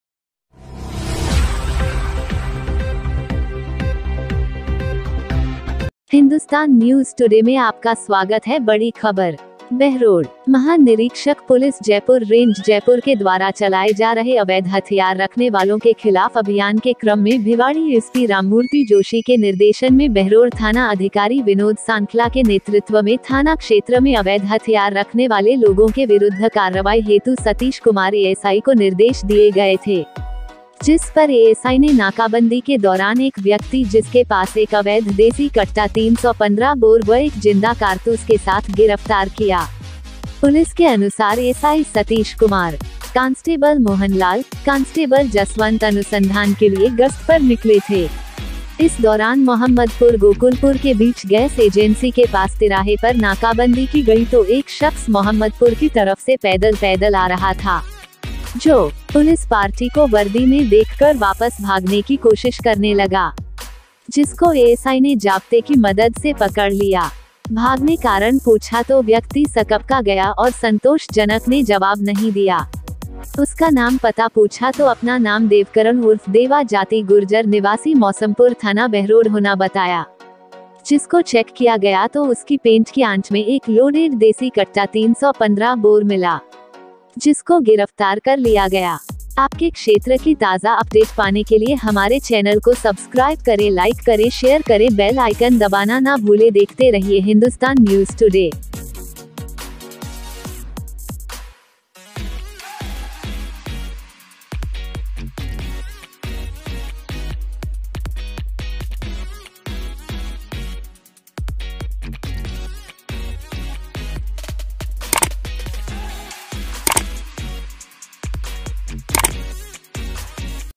बड़ी खबर देसी कट्टे के साथ एक व्यक्ति गिरफ्तार बहरोड़ पुलिस की बड़ी कार्रवाई देसी हथियार के सहित
अवैध देशी कट्टा मय जिन्दा कारतूस के साथ एक ओरापी को किया गिरफ्तार
बहरोड़। महा निरीक्षक पुलिस जयपुर रेंज जयपुर के द्वारा चलाए जा रहे अवैध हथियार रखने वालों के खिलाफ अभियान के क्रम में भिवाड़ी एसपी राममूर्ति जोशी के निर्देशन में बहरोड़़ थाना अधिकारी विनोद सांखला के नेतृत्व में थाना क्षेत्र में अवैध हथियार रखने वाले लोगों के विरुद्ध कार्रवाई हेतु सतीश कुमार एएसआई को निर्देश दिए गये थे। जिस पर एएसआई ने नाकाबंदी के दौरान एक व्यक्ति जिसके पास एक अवैध देसी कट्टा 315 बोर व एक जिंदा कारतूस के साथ गिरफ्तार किया। पुलिस के अनुसार एसआई सतीश कुमार, कांस्टेबल मोहनलाल, कांस्टेबल जसवंत अनुसंधान के लिए गस्त पर निकले थे। इस दौरान मोहम्मदपुर गोकुलपुर के बीच गैस एजेंसी के पास तिराहे पर नाकाबंदी की गई तो एक शख्स मोहम्मदपुर की तरफ से पैदल पैदल आ रहा था। जो पुलिस पार्टी को वर्दी में देखकर वापस भागने की कोशिश करने लगा। जिसको एएसआई ने जाब्ते की मदद से पकड़ लिया। भागने कारण पूछा तो व्यक्ति सकपका गया और संतोष जनक ने जवाब नहीं दिया। उसका नाम पता पूछा तो अपना नाम देवकरण उर्फ देवा जाति गुर्जर निवासी मौसमपुर थाना बहरोड होना बताया। जिसको चेक किया गया तो उसकी पेंट की आंट में एक लोडेड देसी कट्टा 315 बोर मिला। जिसको गिरफ्तार कर लिया गया।